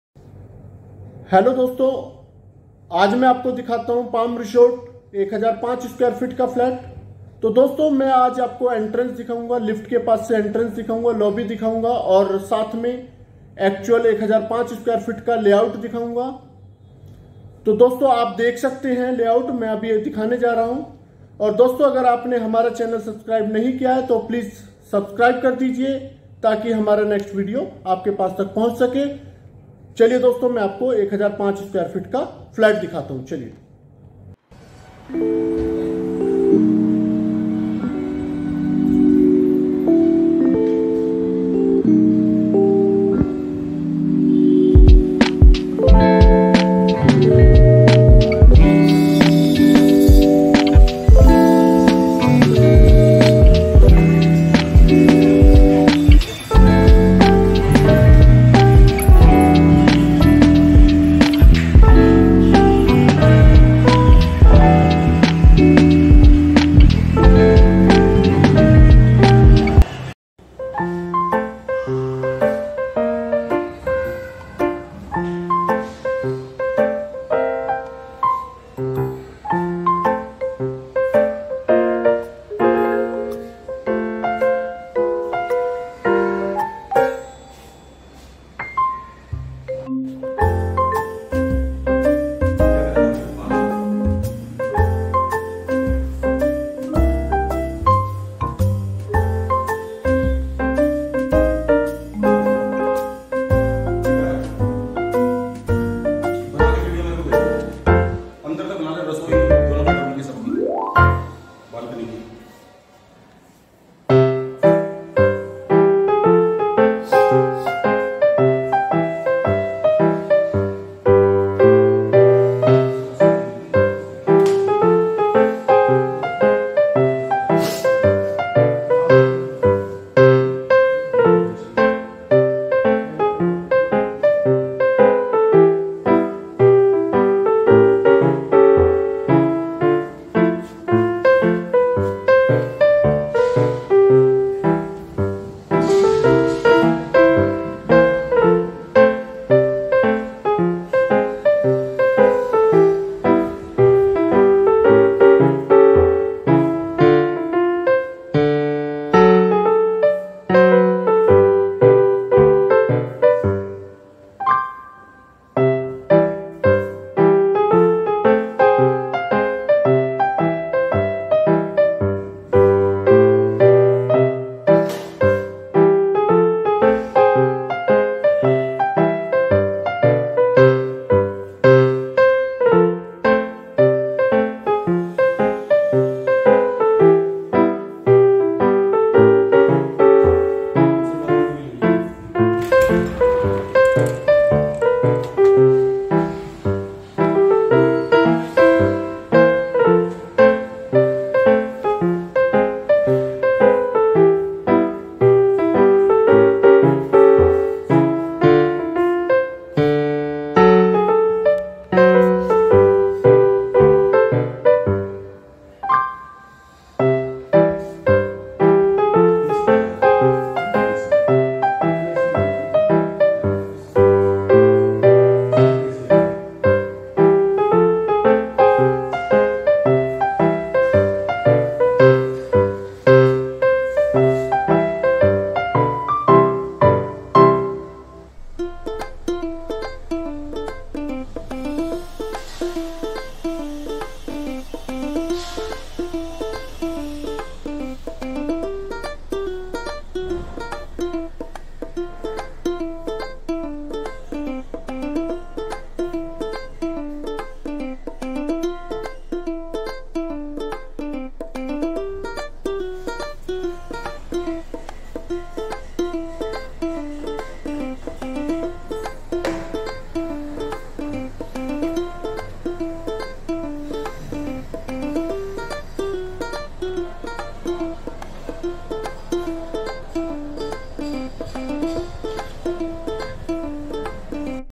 Palm resort 1005sqft flats raj nagar extension Ghaziabad.
Palm resort raj nagar extension Ghaziabad.
2bhk, 3bhk ,4bhk and duplex flats available flats available on raj nagar extension Ghaziabad ready to move project fully facility and prime Location raj nagar extension Ghaziabad.

2bhk
3bhk
4bhk
Duplex

Luxurious project